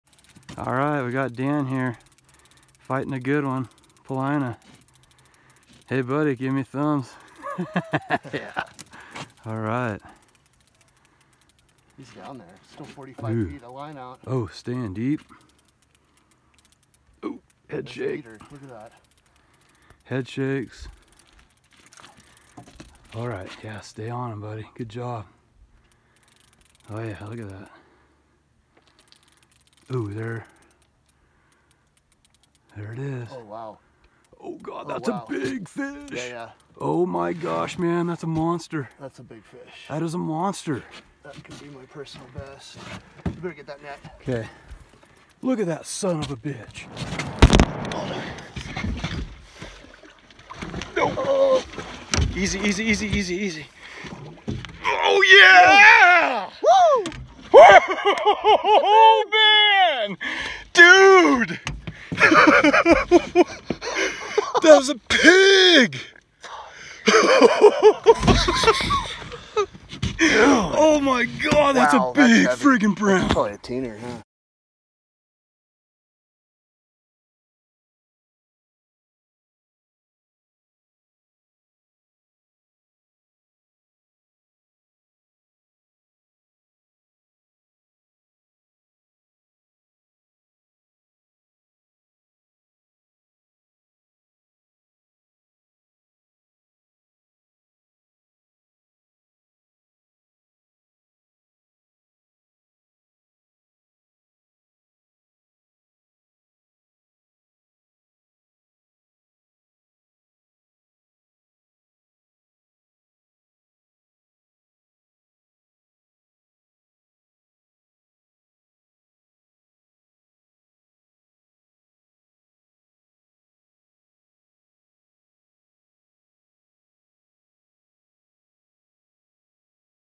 18-1/2 Pound Hen Brown Dream Catch Paulina Lake, OR
Dan McAllister catches his Dream hen from Paulina Lake, OR in mid May of 2020.  This video catches all the action and excitement of the fight!  This gorgeous ole girl was released to fight another day after proper catch and release practices and weights and measurements.  That release video will soon follow!  Climb on board with TTH and enjoy this video!